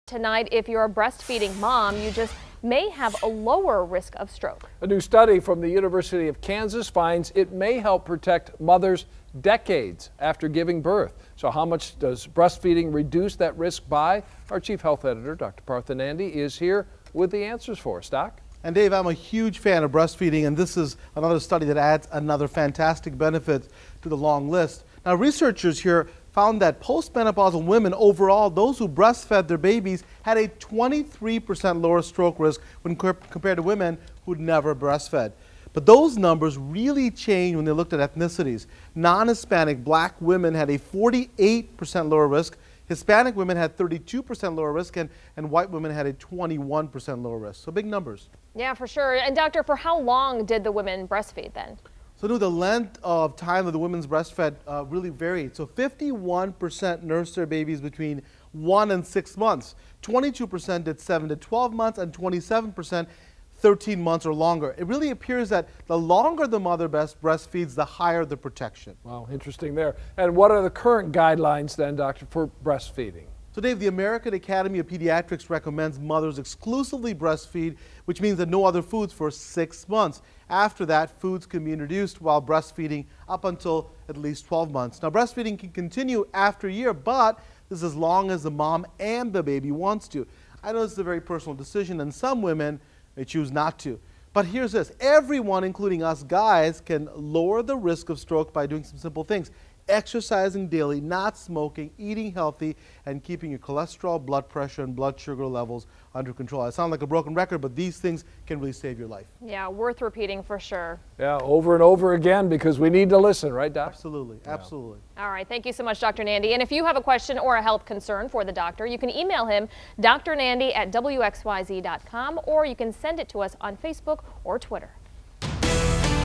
Ask Dr. Nandi: Breastfeeding linked to reduced risk of stroke later in life
If you’re a breastfeeding Mom, you just may have a lower risk of stroke.  A new study from the University of Kansas finds it may help protect mothers decades after giving birth.   I’m a huge fan of breastfeeding and this study adds another fantastic benefit to its list.